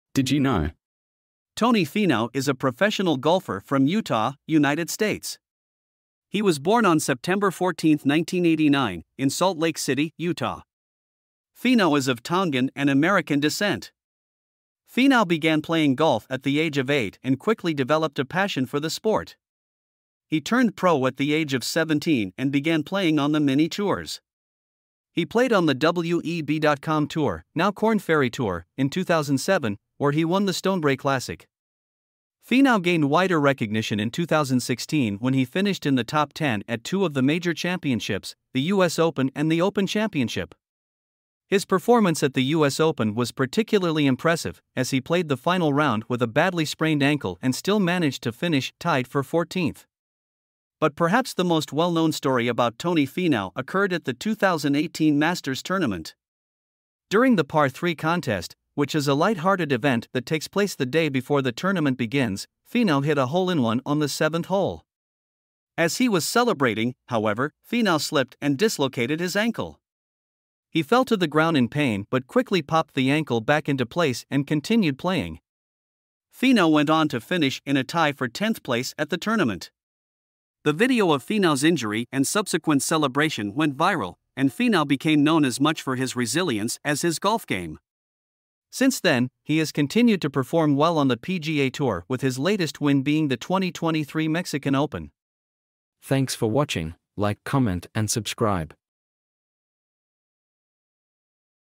Tony Finau's Hole-In-One Disaster: The Par-3 Contest Incident
Follow the incredible journey of Tony Finau, from his humble beginnings on the mini-tours to becoming a major contender on the PGA Tour. This inspiring story showcases Finau's talent, determination, and resilience in the face of adversity